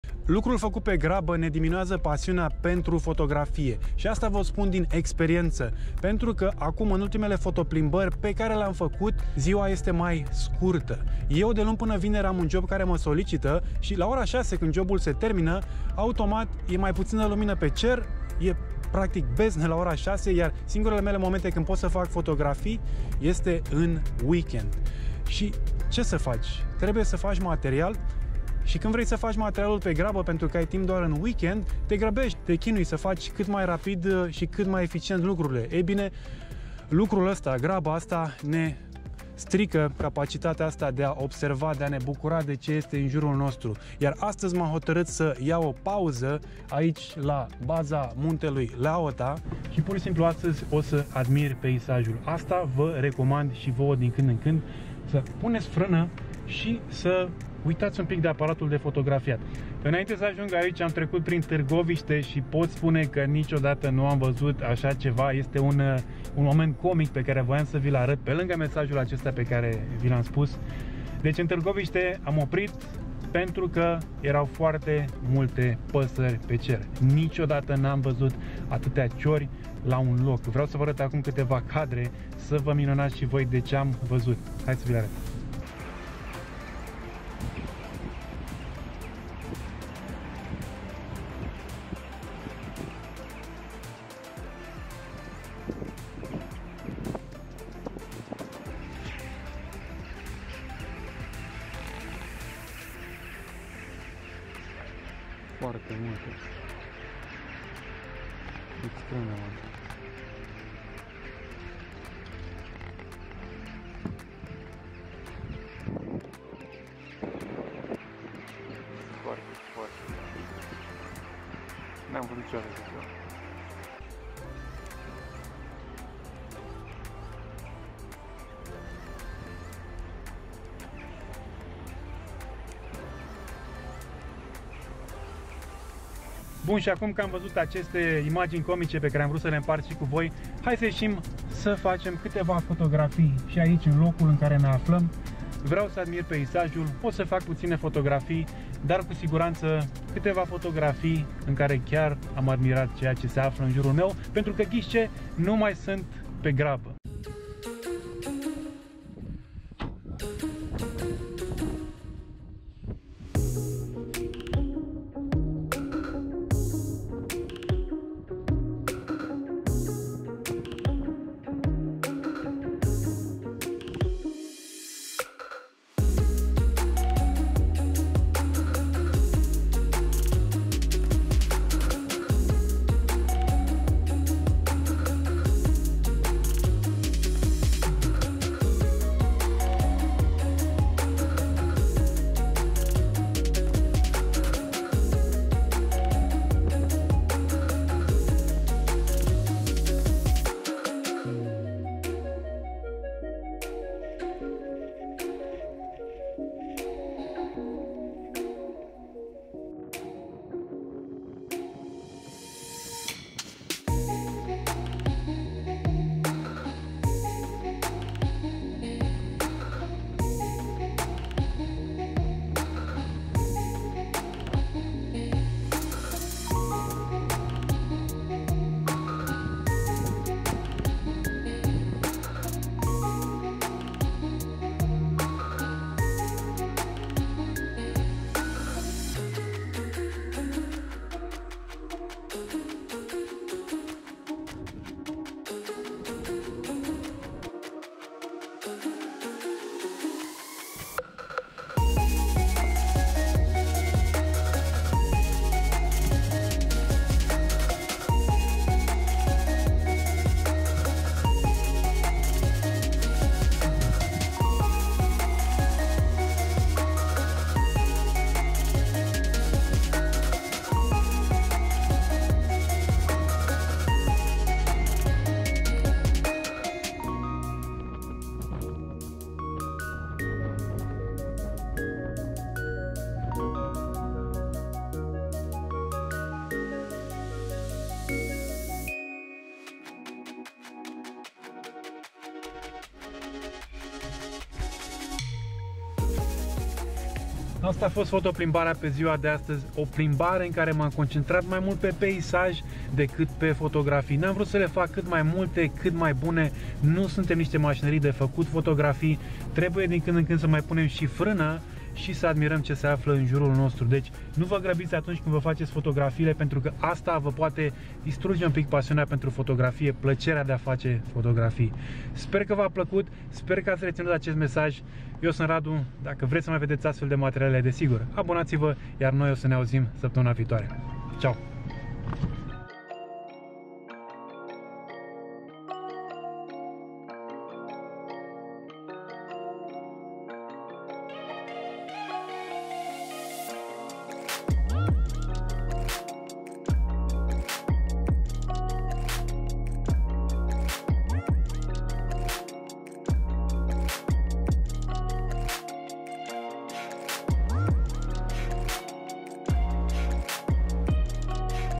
Nu te grăbi la fotografie - Foto Plimbare ep.18
Fotografii la baza Muntelui Leaota într-o scurtă expediție turistică ce mă determină să fac ceea ce nu prea obișnuiesc, fotografie de tip piesaj. Tot în acest clip o să vă spun o lecție pe care am învățat-o recent, utilă oricărui fotograf .